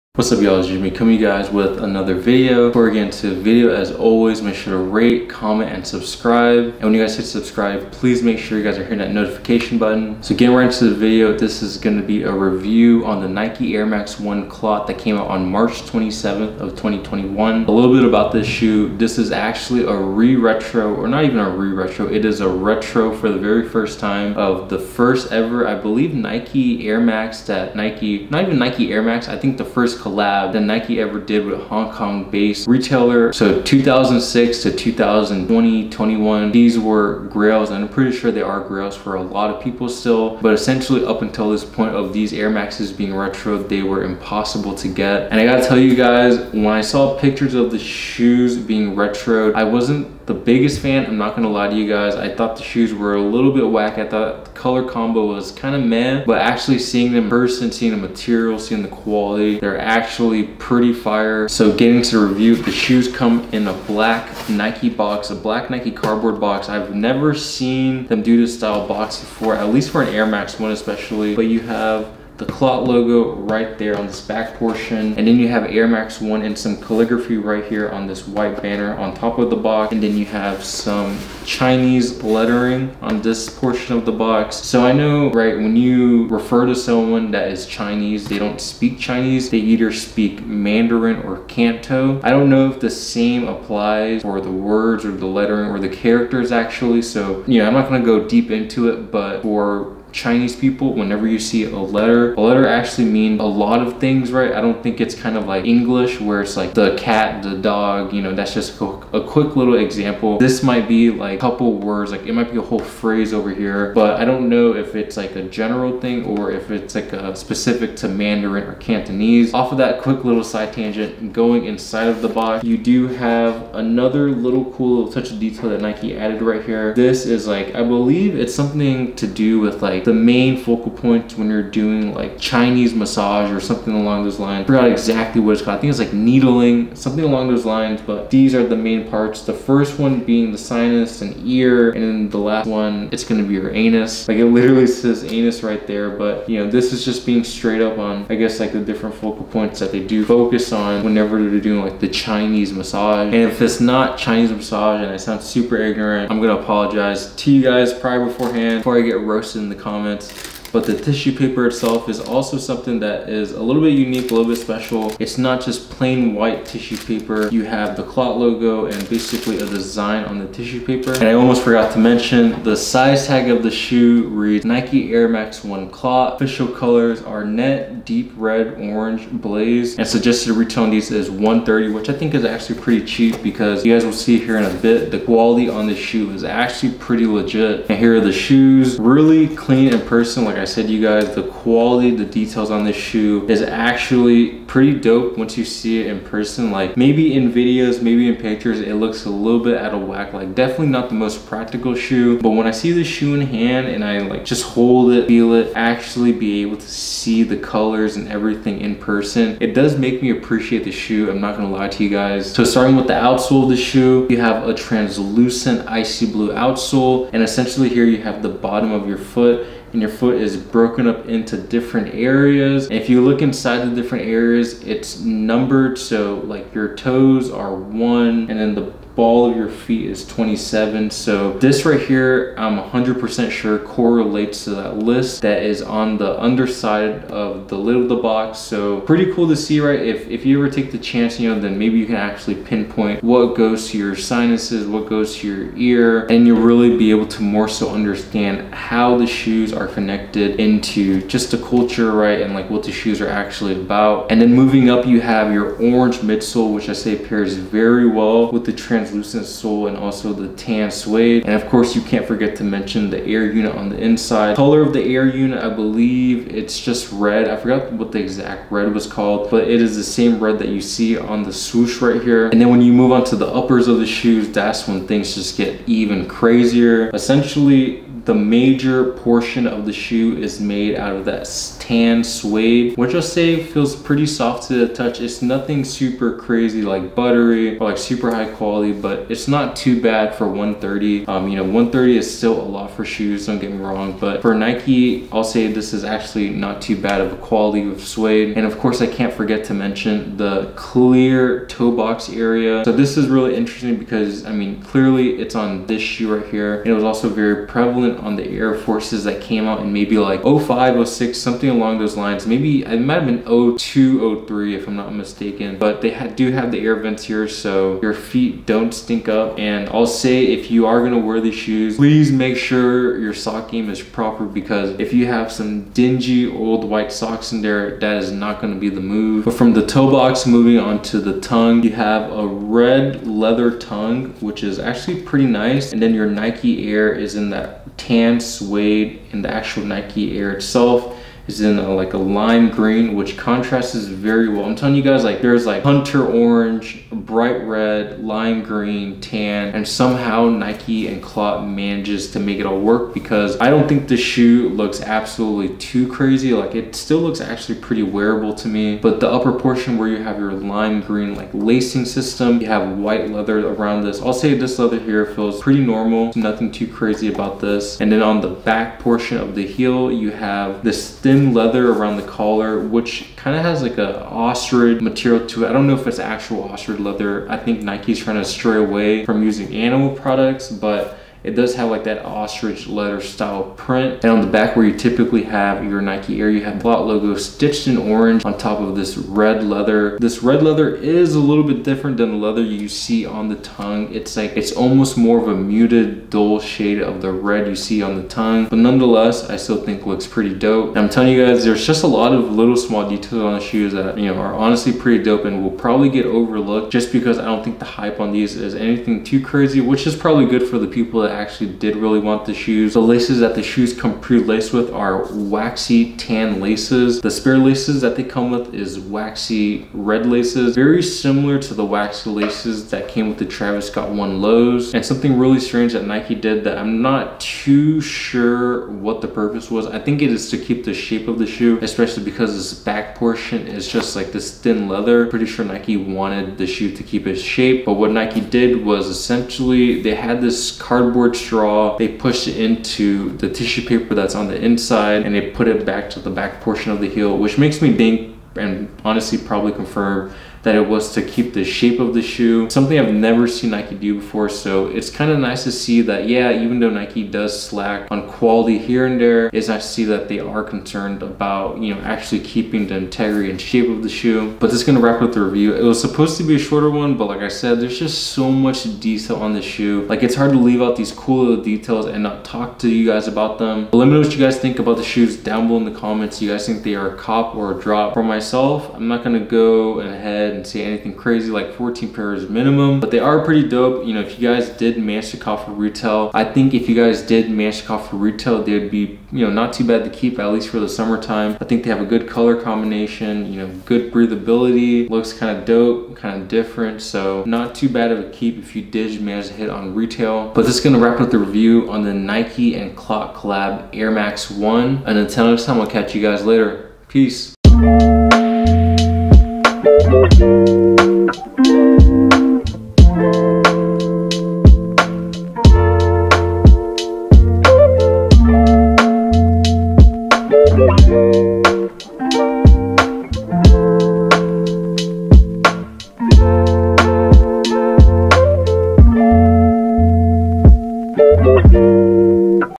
Nike Air Max 1 Clot "Kiss of Death" Review + Detailed Look
Today we're checking out the Nike Air Max 1 Clot "Kiss of Death" that released on 03/37/2021! Do you guys think these are a cop or a drop?